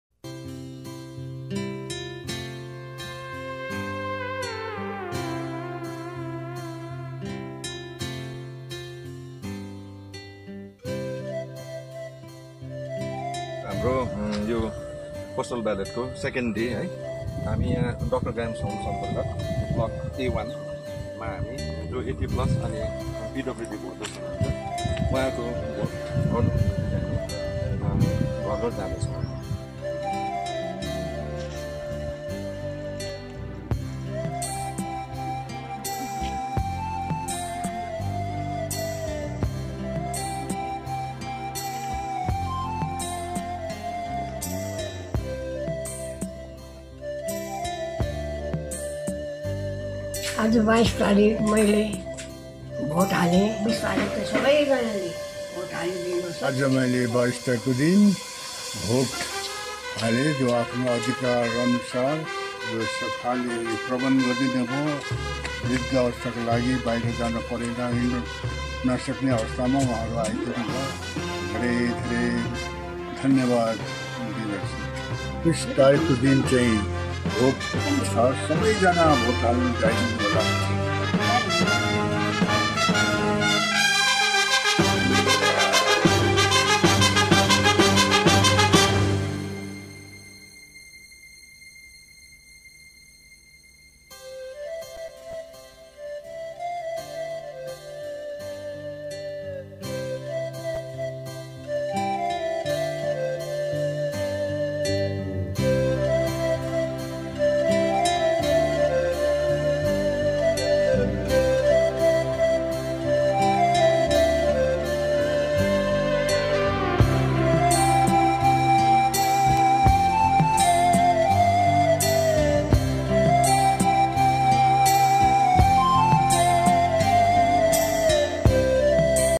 Home Voting going on at different areas of Kalimpong district
Glimpse of Home Voting going on at different areas of Kalimpong district, West Bengal.